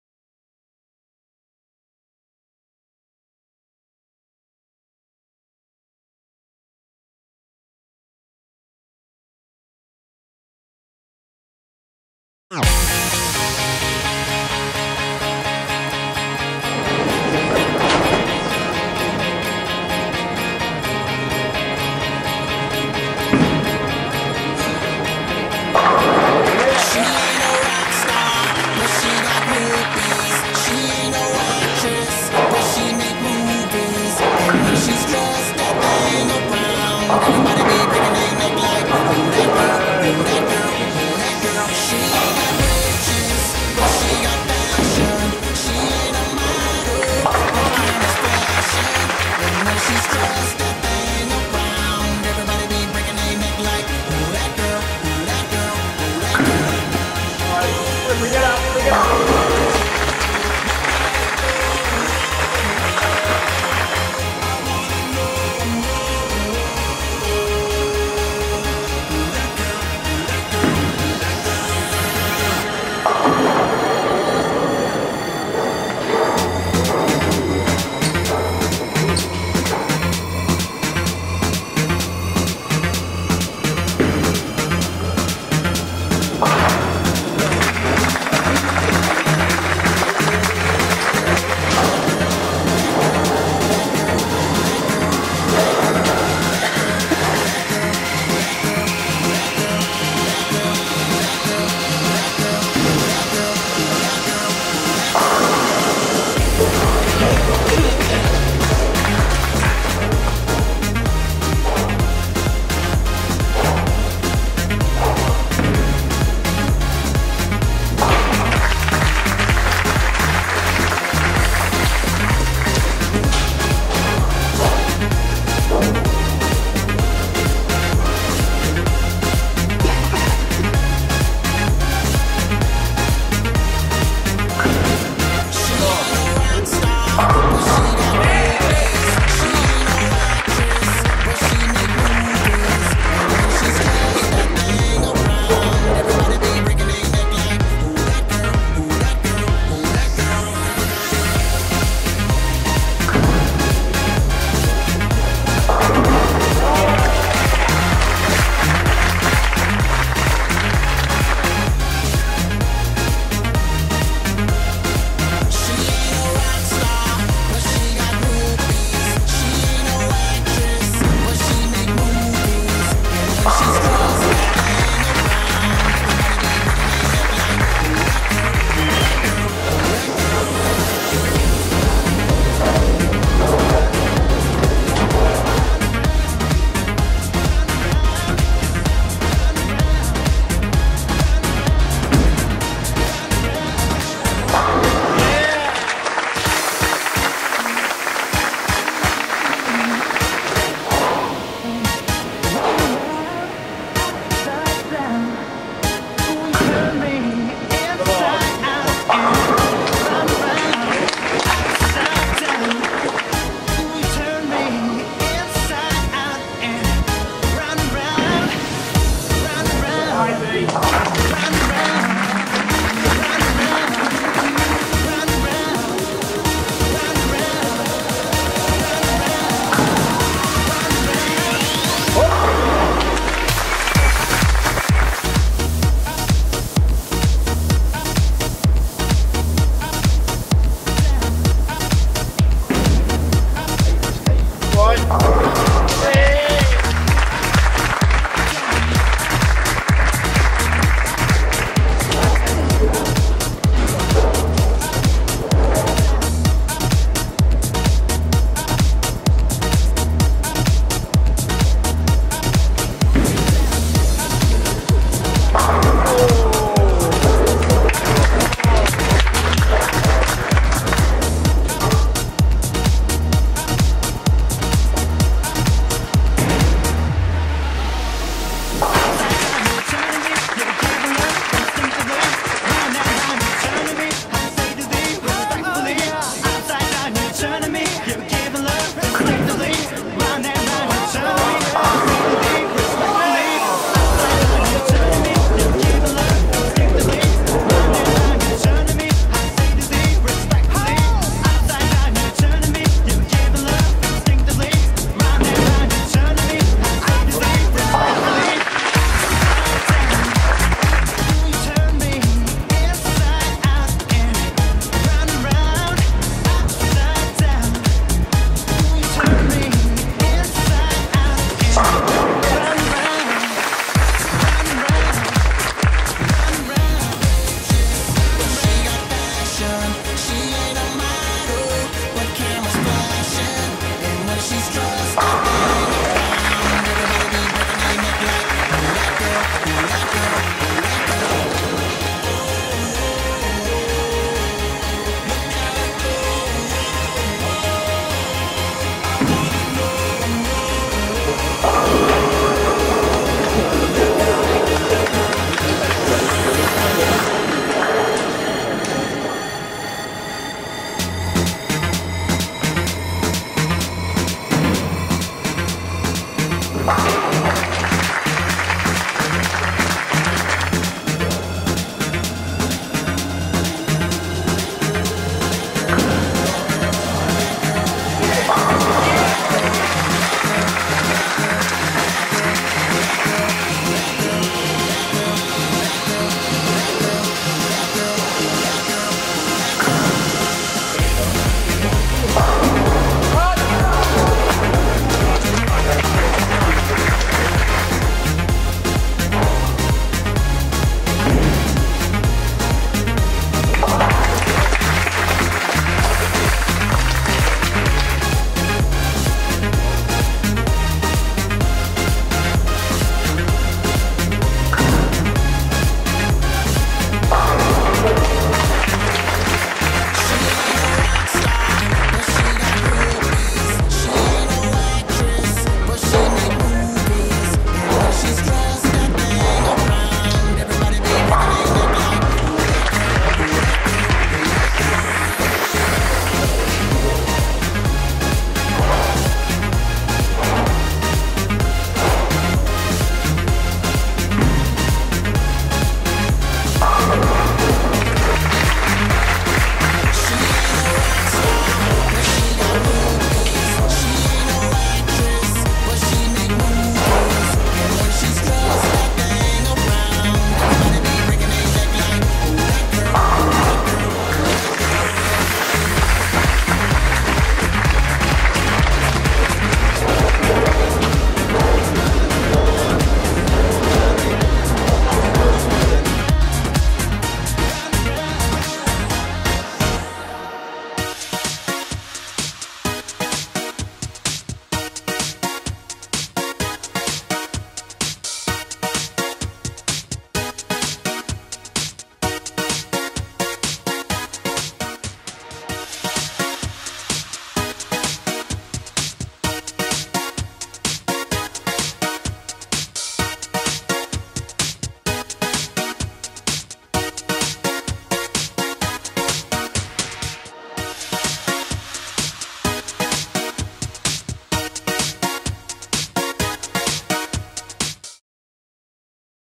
2011QubicaAMFWorldCup - GAMEII - Women'sFinal.mov
The 69 women, who participated in the 47th QubicaAMF Bowling World Cup at Northcliff Bowling Center in Johannesburg, South Africa, bowled 20 qualifying games, four blocks of five games on four days, before the field was cut to the top 24 who bowled eight additional games.
The top eight after 28 games advanced to the Round of 8 and bowled another eight games on to cut to the top three who will determine the champion in the stepladder (best 2 out of 3) finals on Wednesday, Dec. 7, 2011.
Finals GAME II: Tides turned in game two, when No. 2 seed Asbaty (USA), who was targeting for her second career World Cup title after her victory in Caracas Venezuela in 2006, rebounded with 227-214 win to level the match against No. 1 seed Guerra (Dominican Republic).